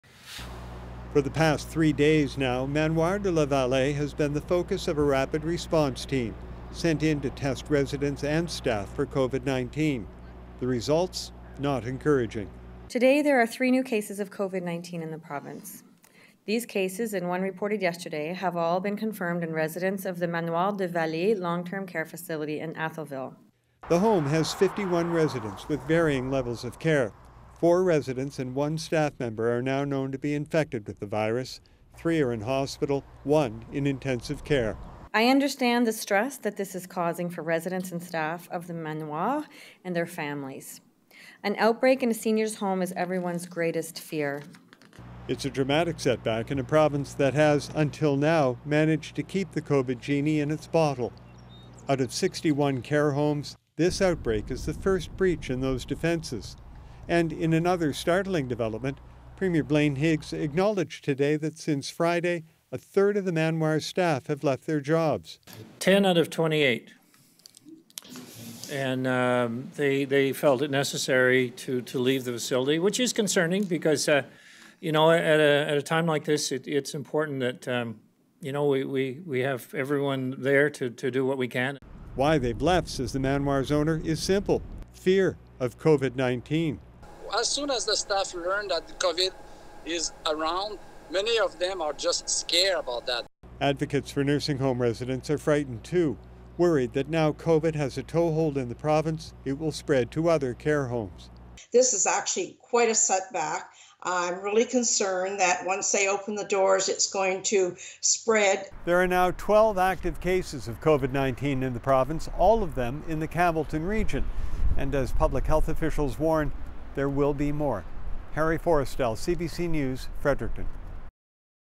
5 COVID-19 cases in long-term care home linked to N.B. doctor
Five cases of COVID-19 in a Campbellton, N.B., long-term care home — four residents and an employee — are linked to a local doctor who travelled to Quebec and didn’t self-isolate afterward.